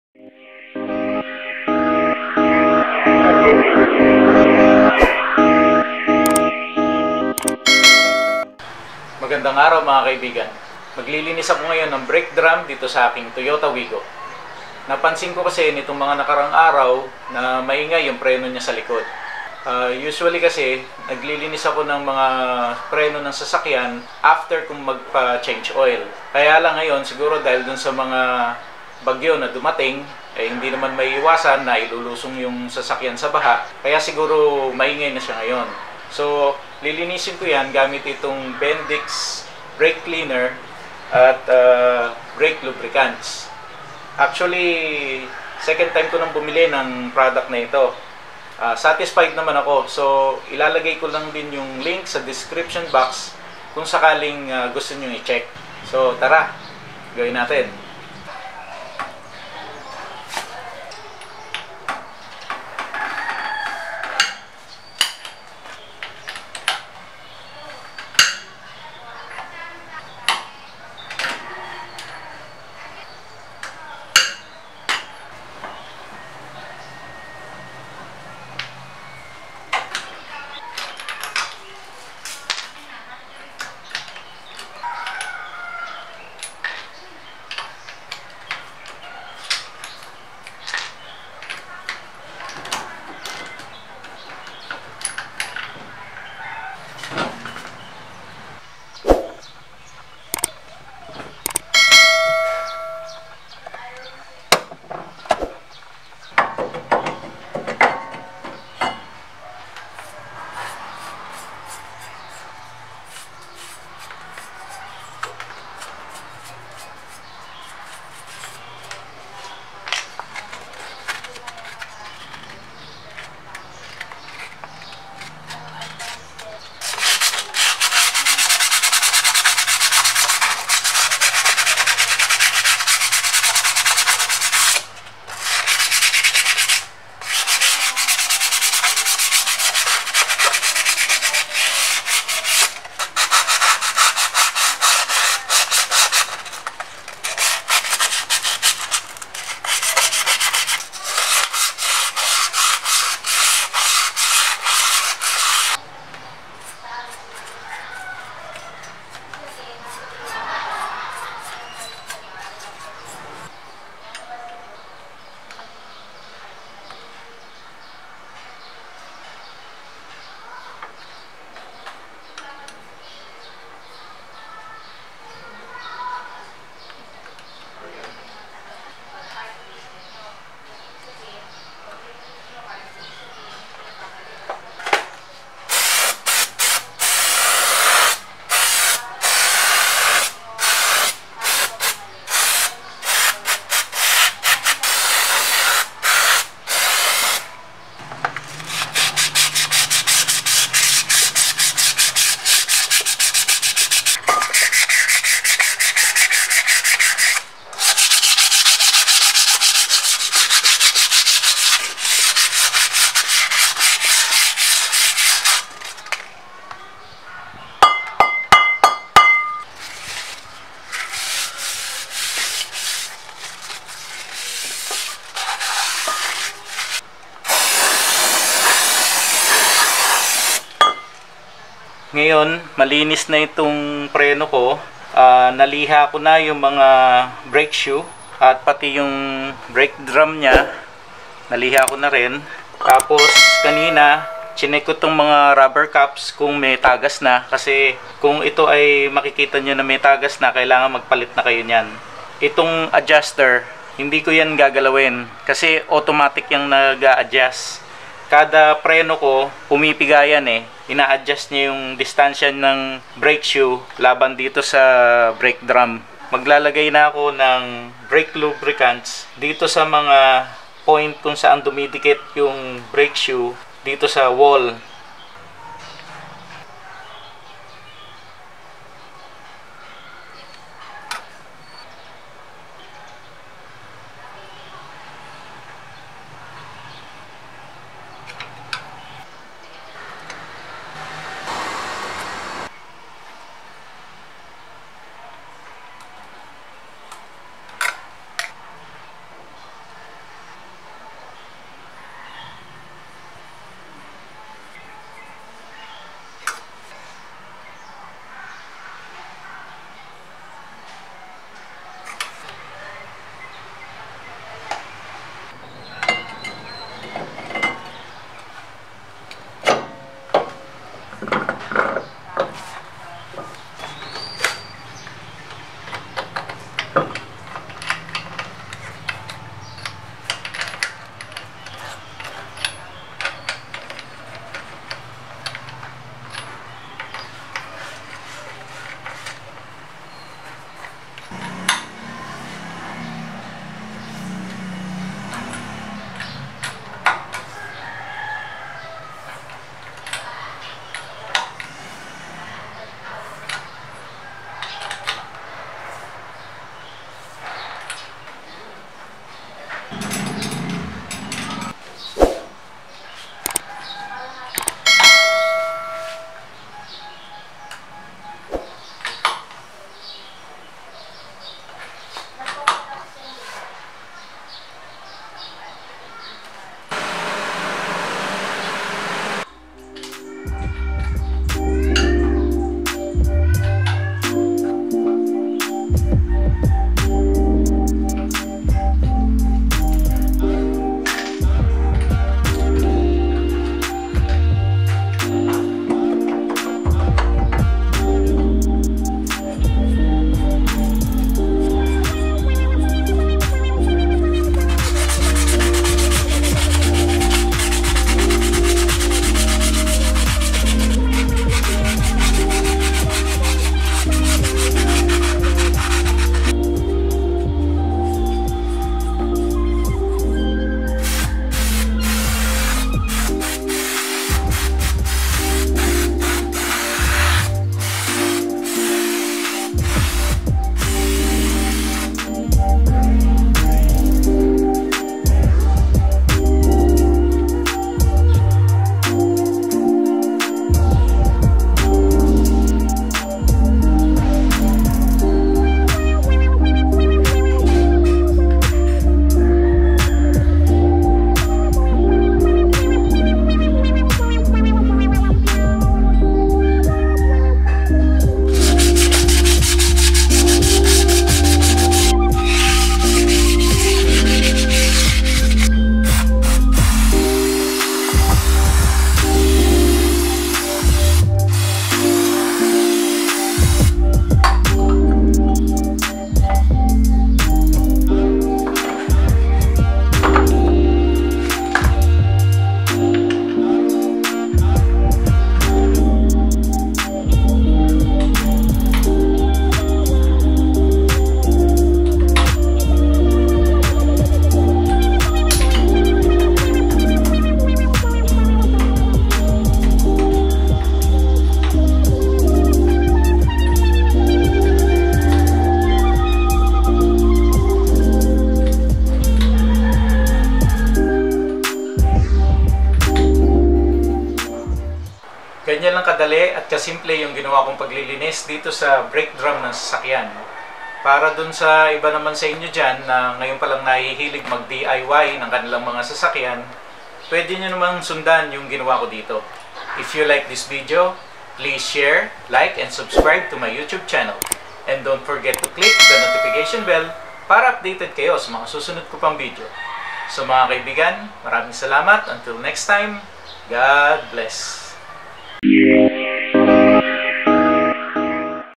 How To Clean Drum Brakes, Fast and Easy on my Toyota Wigo
This is the link to the brake cleaner and lubricant.

Hello! I'm Joey. In this video, I will share with you how I cleaned the drum brakes of my 2015 Toyota Wigo, using brake cleaner for a fast and simple clean. A good video for beginner DIY'ers who always wants their brakes clean.